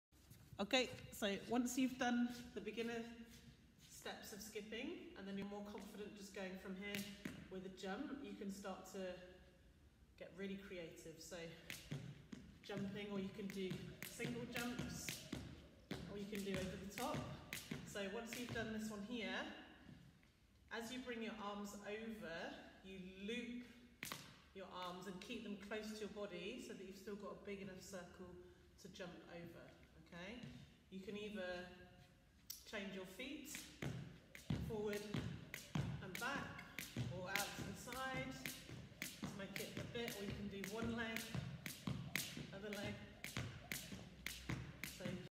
Skipping Challenges
How to keep kids entertained indoors with fun challenges